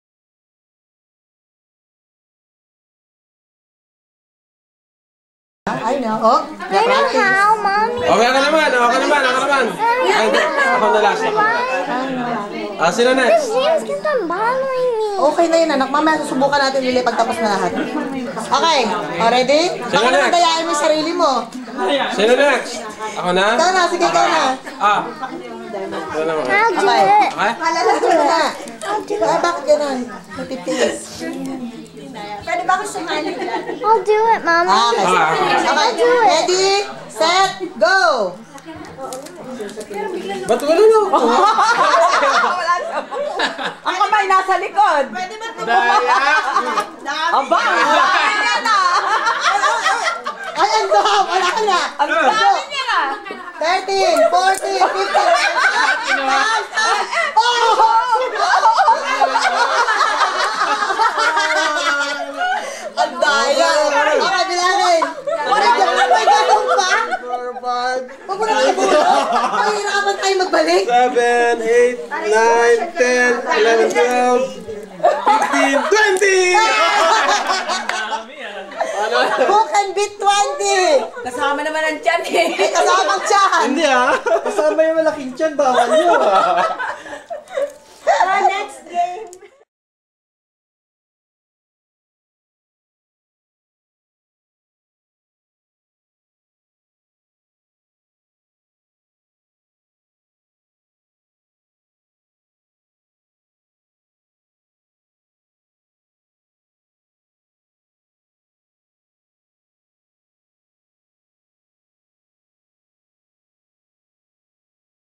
Clothes Pin Game
Allan wins the clothes pin game.  When we went looking for evidence, eto ang nakita namin!  hmmm  I smell something fishy...  ay may katabi pala akong isda :)